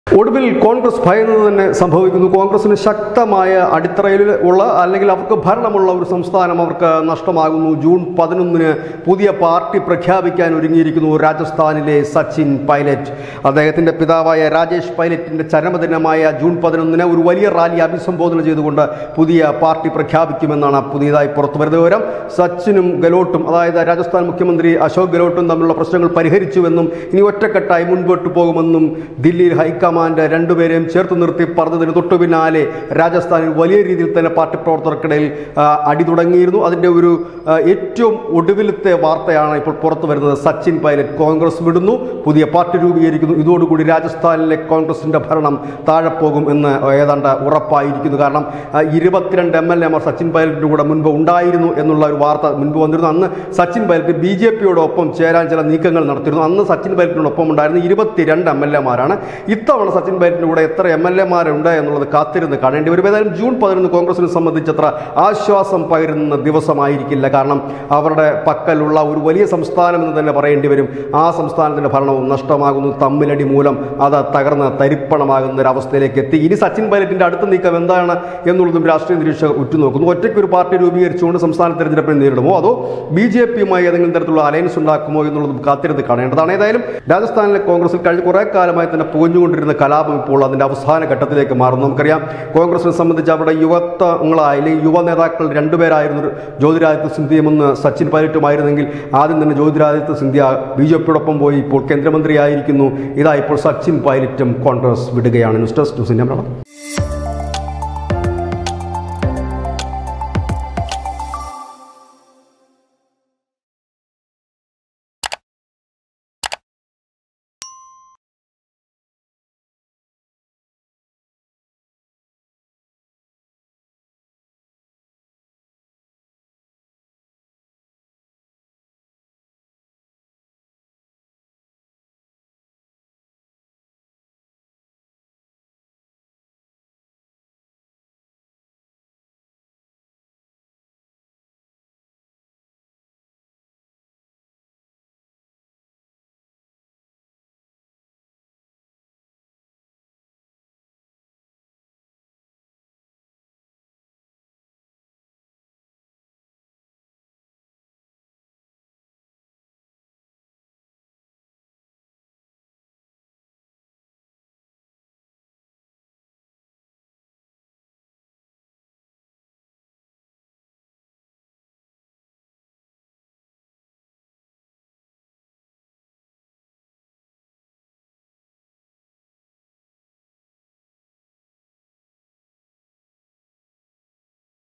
അനുനയങ്ങള്‍ പാളി I NEWS INDIA MALAYALAM
കോണ്‍ഗ്രസ്സ് ഭയന്നത് സംഭവിക്കുന്നു!
പിളര്‍പ്പിലേക്ക്!!
അനുനയങ്ങള്‍ പാളി...